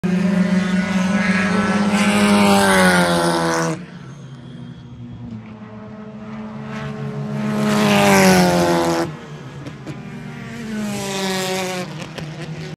Cash and Roman Felber F4 Testing at Homestead Miami Speedway
Cash and Roman Felber F4 Testing at Homestead Miami Speedway in January 2024 preparing for the FARA Formula Championship.